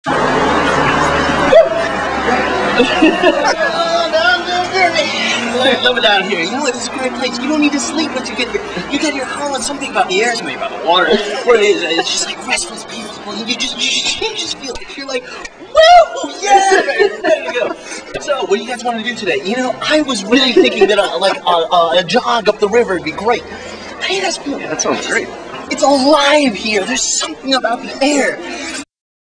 German Coffee Interview: MOJO & The Bayou Gypsies' Fiddler
MOJO & The Bayou Gypsies' Fiddler, Greg Hirte ("The Fire), speaks about the wonders of German coffee, life in Germany, and the fun the band had during  their German Tour (Stimmen Fest, 2007).  Shot in Lorrach, Germany.  Fueled by Cappuccino Caffeine.